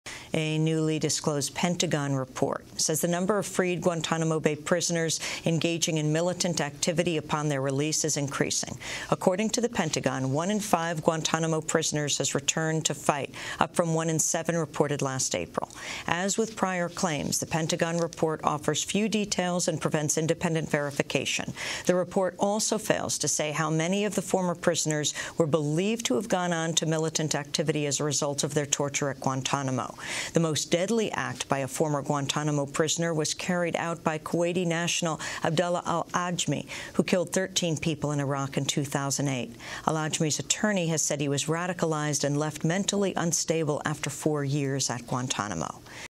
DN! 20% of Freed Gitmo Prisoners Engage in Militant Activity
Pentagon: 20% of Freed Gitmo Prisoners Engage in Militant Activity

A newly disclosed Pentagon report says the number of freed Guantanamo Bay prisoners engaging in militant activity upon their release is on the rise. On Wednesday, the Pentagon says one in five Guantanamo prisoners has returned to fight, up from one in seven reported last April. As with prior claims, the Pentagon report offers few details and prevents independent verification. The report also fails to say how many of the former prisoners were believed to have gone on to militant activity as a result of their torture at Guantanamo. The most deadly act by a former Guantanamo prisoner was carried out by Kuwaiti national Abdallah al-Ajmi, who killed thirteen people in Iraq in 2008. Al-Ajmis attorney has said he was radicalized and left mentally unstable after four years of torture at Guantanamo.